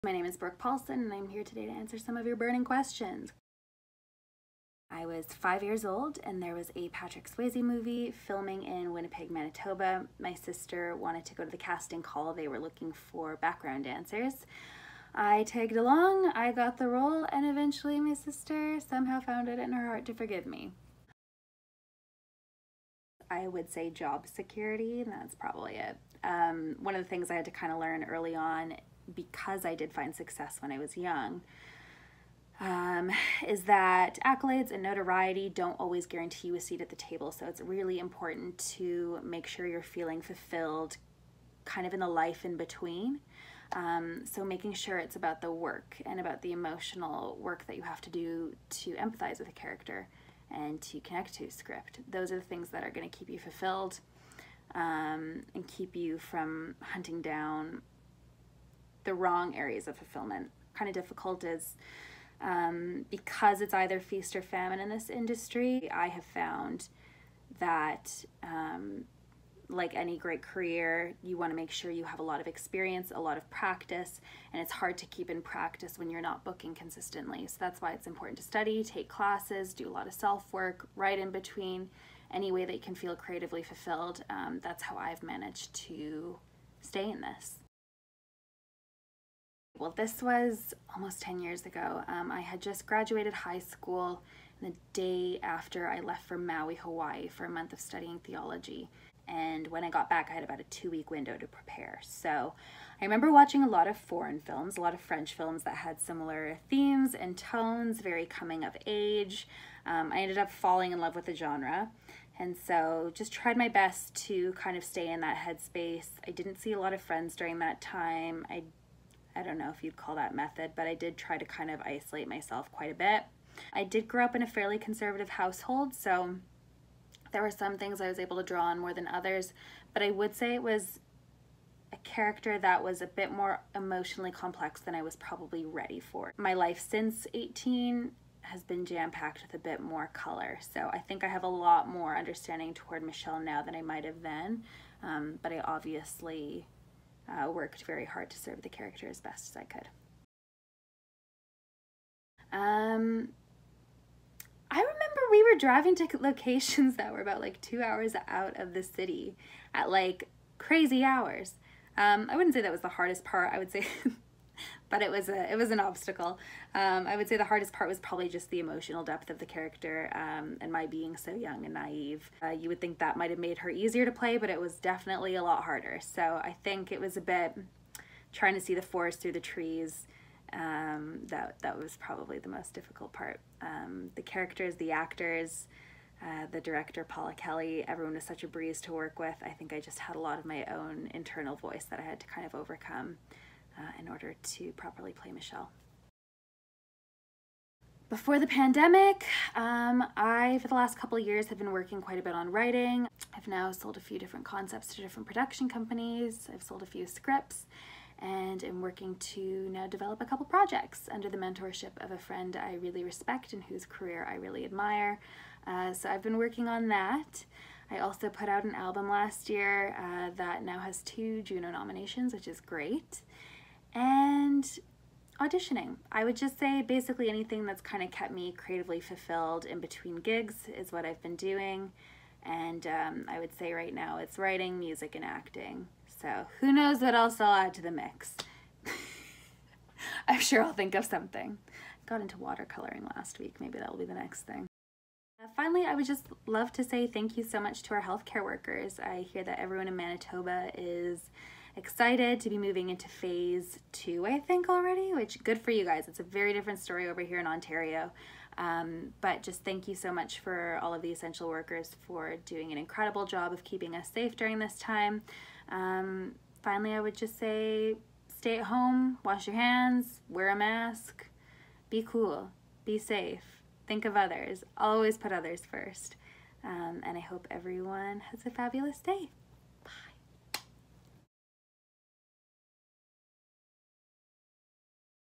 Euphoria Cast Q&A with lead actor Brooke Palson
If you have not seen it on our Instagram page yet, check out this Q&A from Brooke Palson the lead actor in the 2013 drama Euphoria.